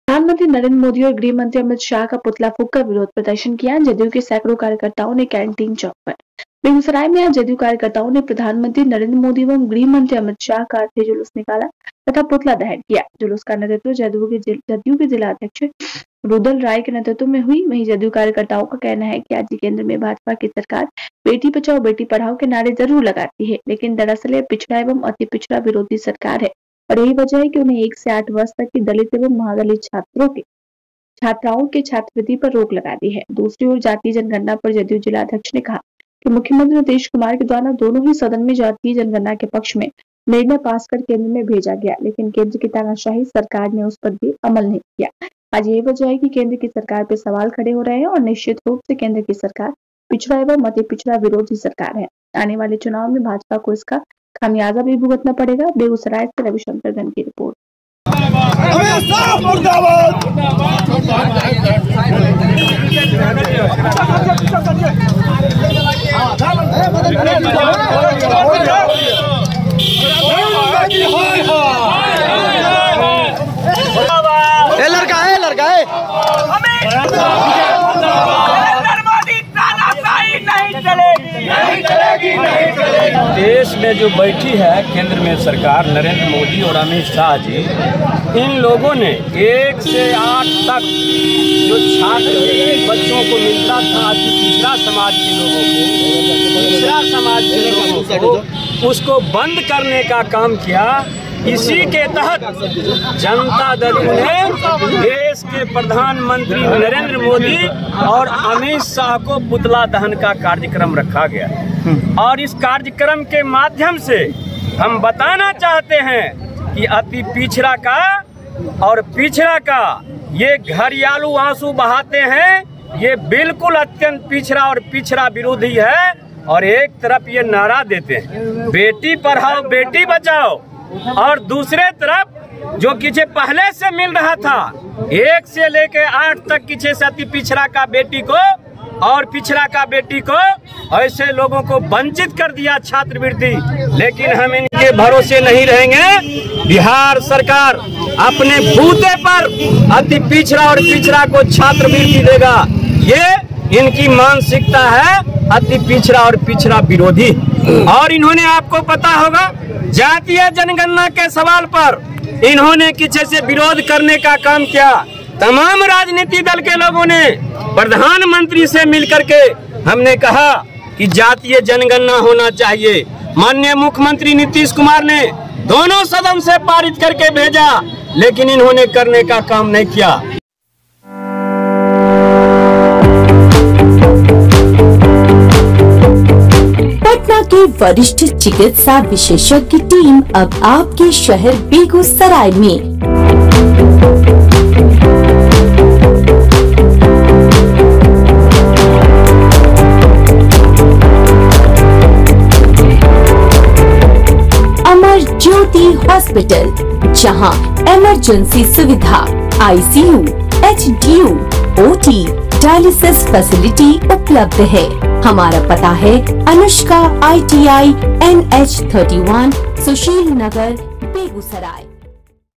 प्रधानमंत्री नरेंद्र मोदी और गृह मंत्री अमित शाह का पुतला फूंककर विरोध प्रदर्शन किया जदयू के सैकड़ों
प्रधानमंत्री नरेंद्र मोदी और गृह मंत्री अमित शाह का पुतला फूंककर विरोध प्रदर्शन किया जदयू के सैकड़ों कार्यकर्ताओं ने केंटीन चौंक पर।btnewsbihar